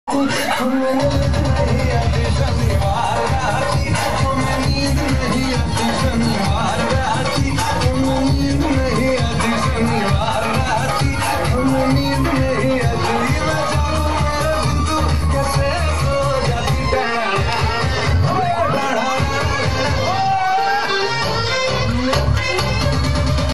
Patients suffer wedding party in the premises of a Bulandshahr hospital | ippodhu.com
Patients suffer as staff attends wedding of an employee's daughter in the premises of a Bulandshahr hospital | ippodhu.com